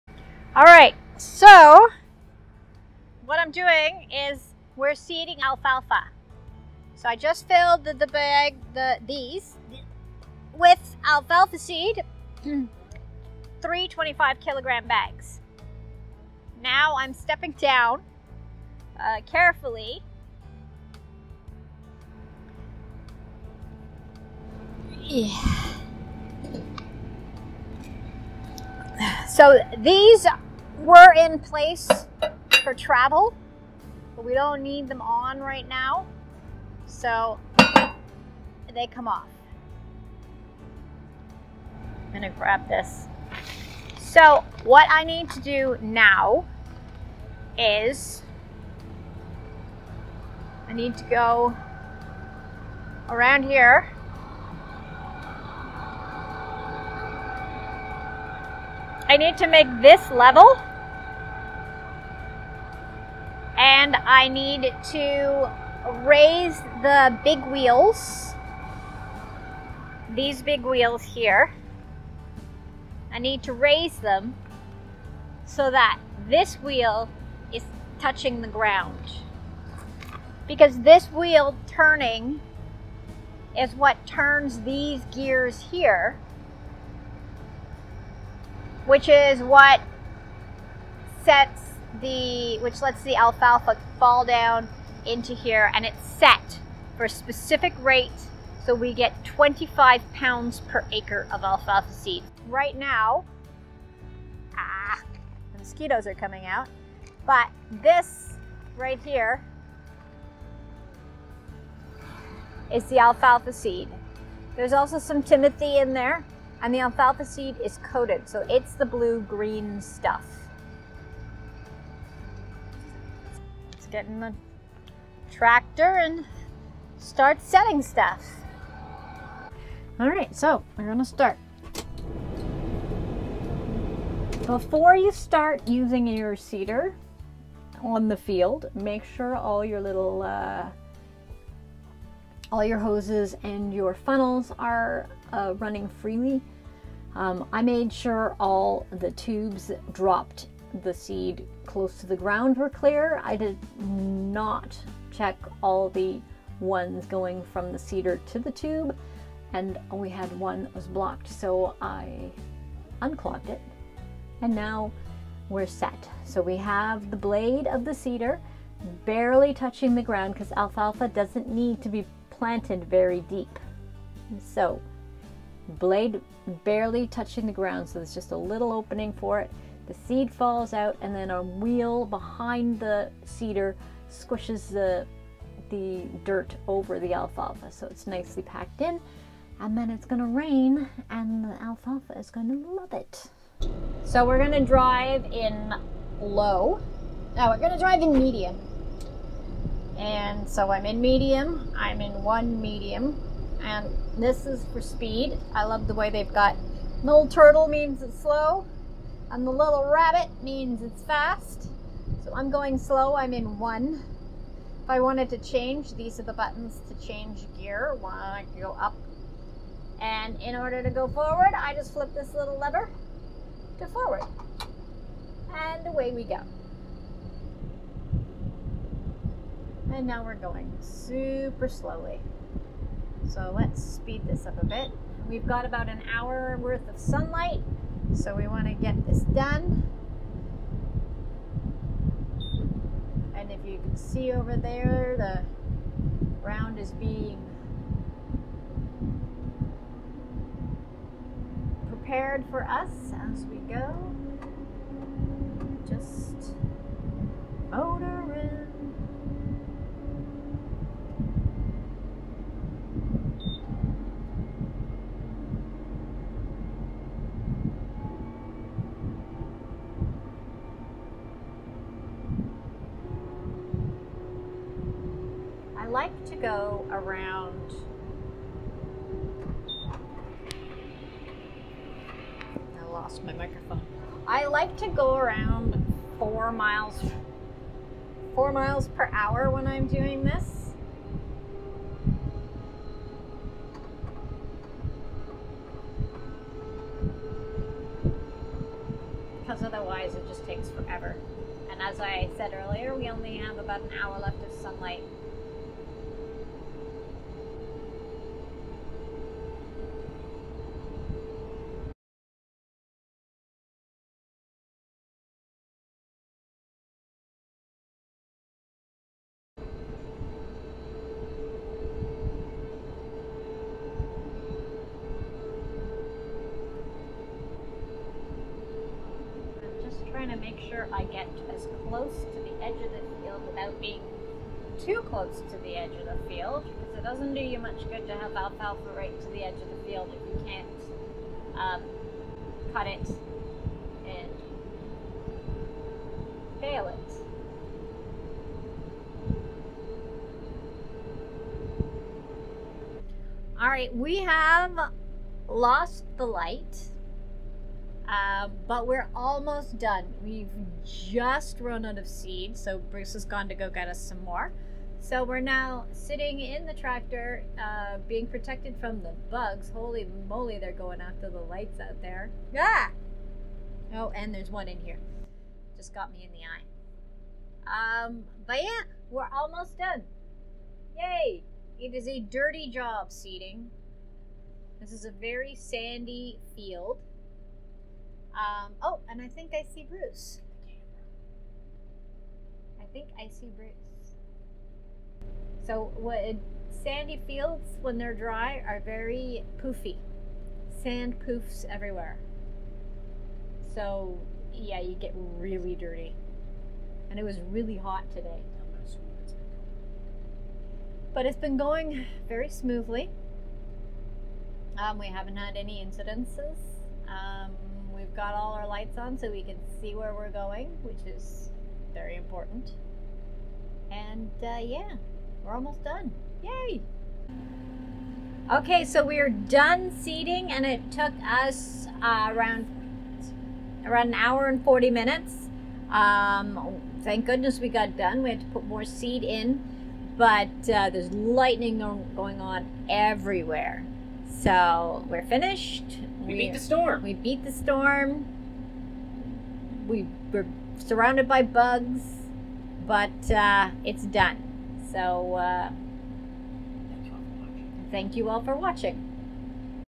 Operating a Remlinger 2300 No-till Drill
In this video we operate our Remlinger 2300 No-till Drill. This is a video I've had on my computer for a while. My mother is prominently featured. In this video we are seeding one of our fields with coated alfalfa.